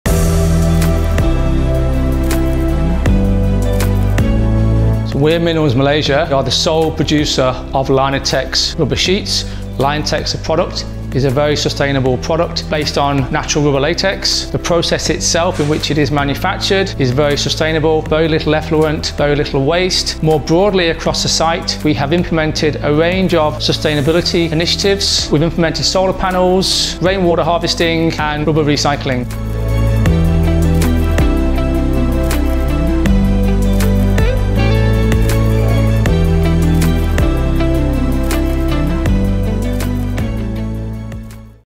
Le caoutchouc Linatex® Premium - 100 ans de durabilité
Le caoutchouc Linatex® Premium, leader des revêtements de protection pour les applications abrasives, célèbre 100 ans de performances et de fiabilité exceptionnelles.